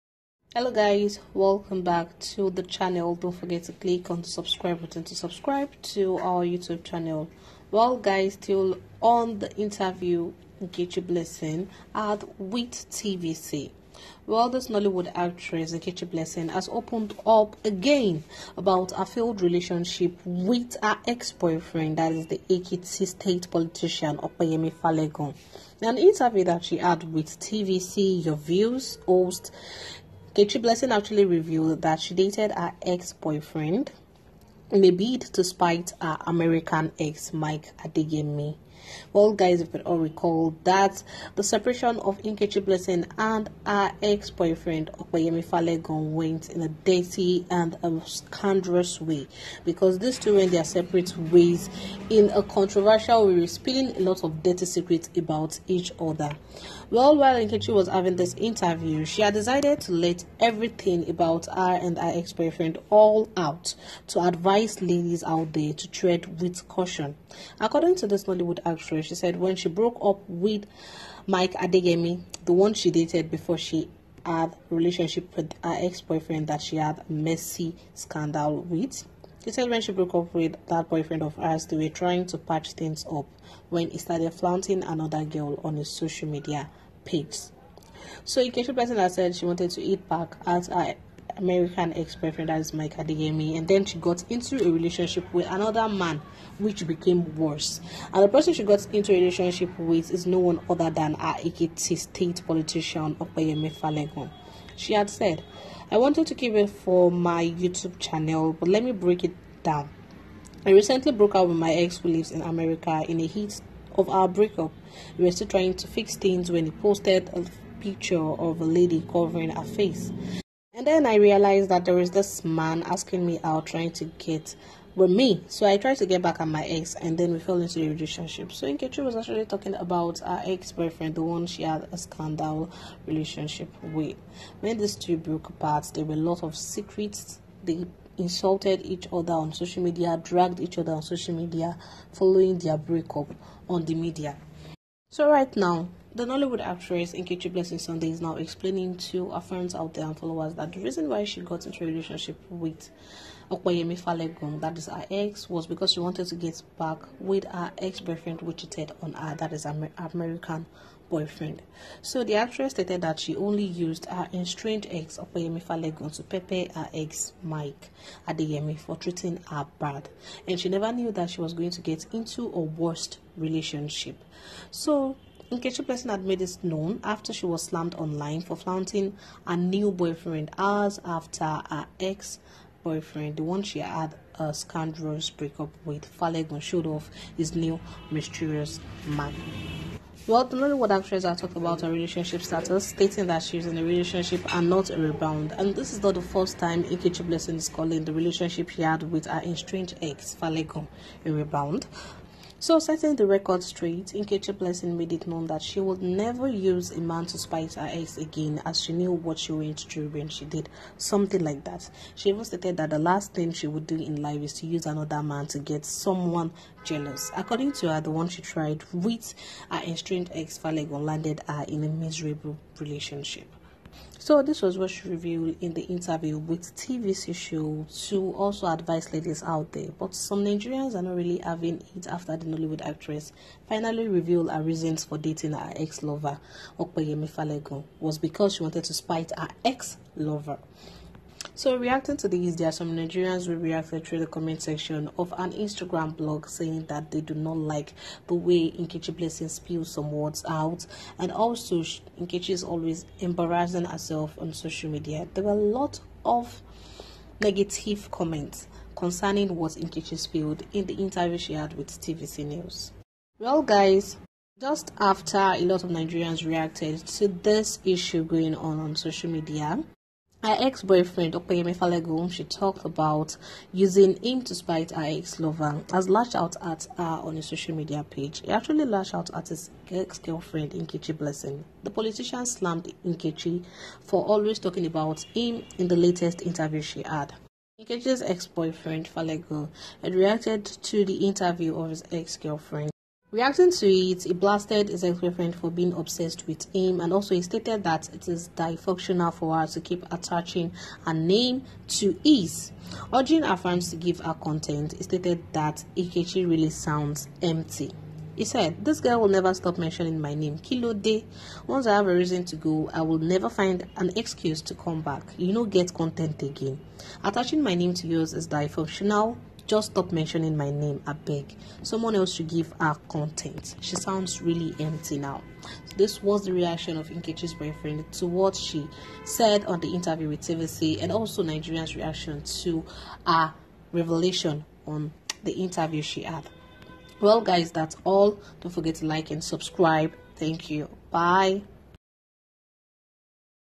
"She is shameless" Neitizens reacts as nkechi blessing reveals reason she dated her estranged......
Nollywood actress Nkechi blessing spill reasons she dated her estranged lover and this got her a lot of negative